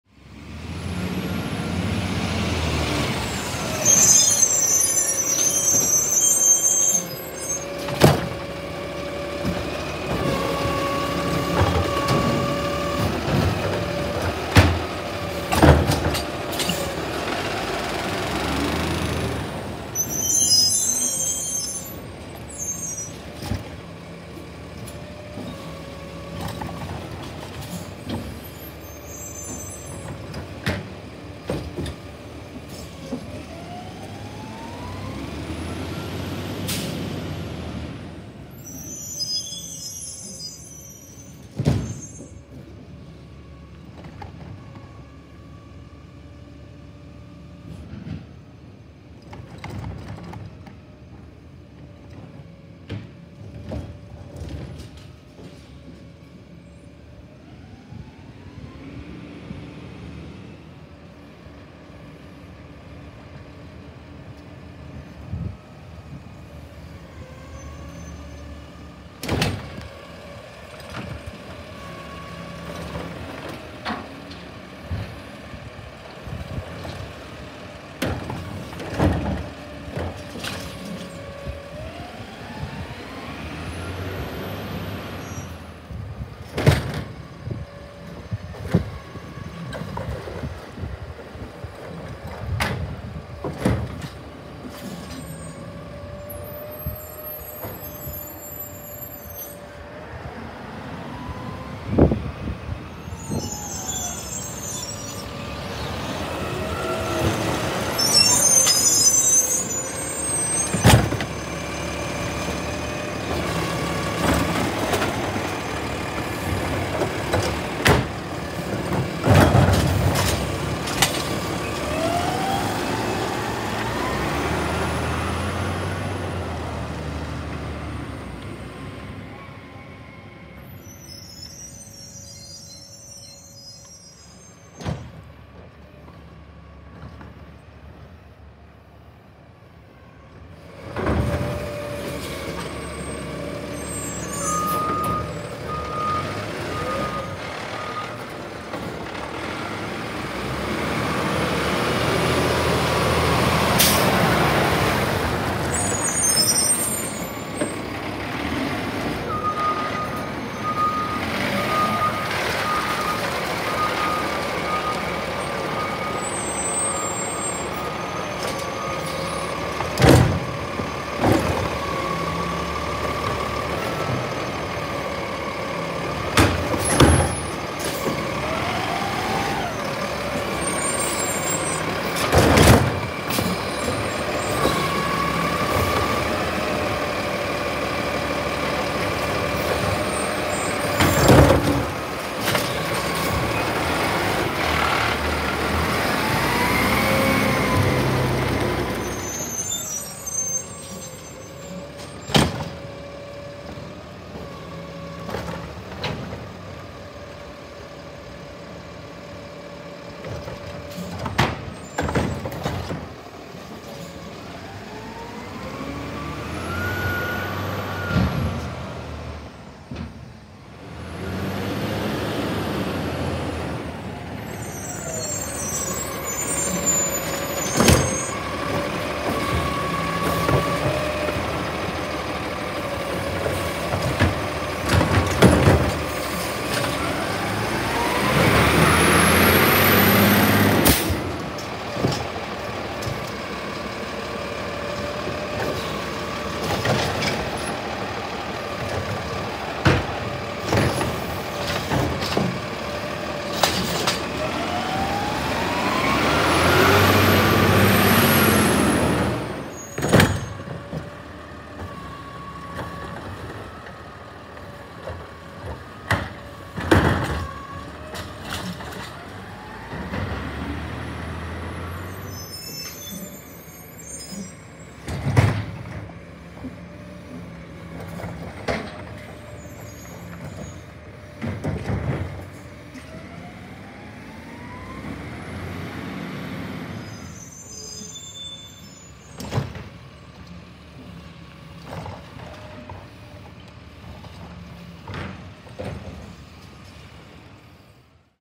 GFL Environmental | Brand New Autocar ACX/ Heil DuraPack Python ASL Garbage Truck
Here’s a pretty fast ACX Python doing trash in Cabot. This is my first video of a GFL truck on route, and I got to film it in different angles. The Ex-WCA Rehrig EGs were really cool, too. I also filmed a WCA Mack LEU E-Z Pack Hercules ASL on recycling, and I’ll have that video up by the next few weeks! Our next trip will be Searcy, and I’ll be filming their trucks on route! This was an amazing truck to film, and I hope y’all enjoy it!

Huge thanks to very friendly driver for letting me film!

To GFL Environmental: Please do not use this video to get any drivers/ employees in trouble.